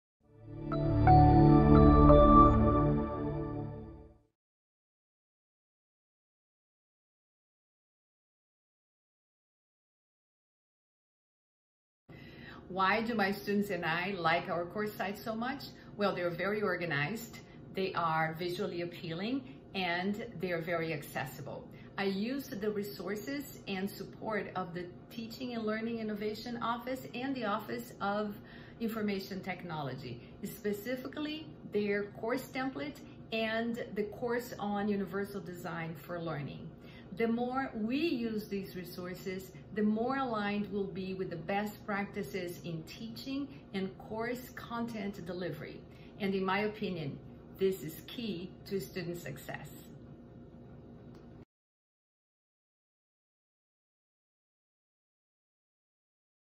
Organizing Your Course Site with the Support of Campus Resources (Just in Time Teaching Tip #1)
Wanessa Velloso of the Department of Modern Foreign Languages & Literatures shares their experience with “Organizing Your Course Site with the Support of Campus Resources,” used during the fall 2020 semester.

For more information on ways to implement this and related effective teaching strategies from UT faculty,